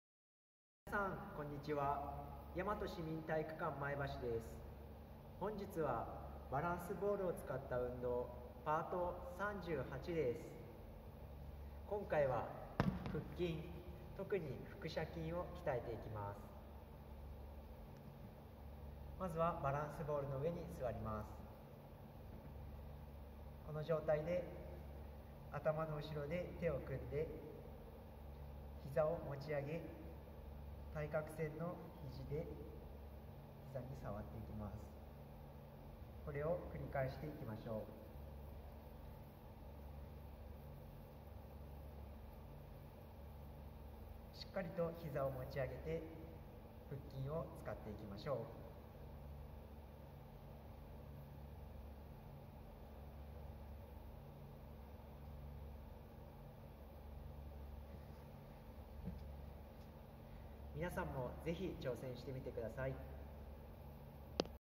バランスボールを使った運動part38
今回は腹筋（特に腹斜筋）を鍛えるトレーニングです。
バランスボールに座り、頭の後ろで手を組みます。
片脚を持ち上げ、対角のひじで膝に触ります。
これを交互に繰り返します。膝をしっかりと持ち上げて腹筋の収縮を感じながら行います。
片側10～15回程度やりましょう。
皆さんもぜひ挑戦してみてください。